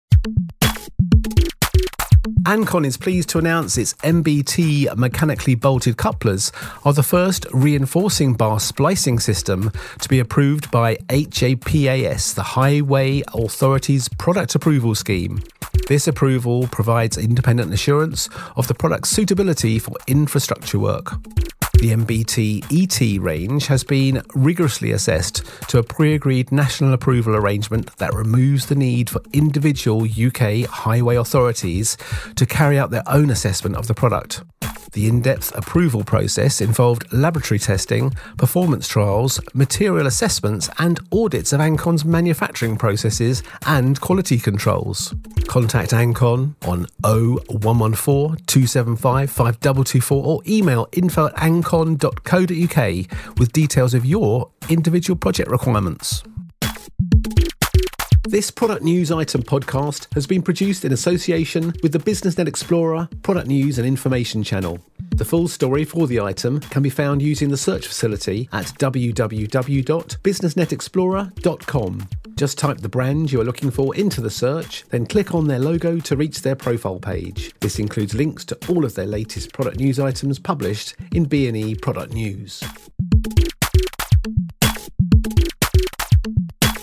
Reinforcing Bar Couplers | Ancon - HAPAS Approval
Ancon product news item published on BusinessNet Explorer and included in BNE Product News - Edition 2 - 2016 | Podcast, Building, Industry, Sector, Specifiers, Buyers, Structural Fixings. For full product information please refer to brand manufacturer.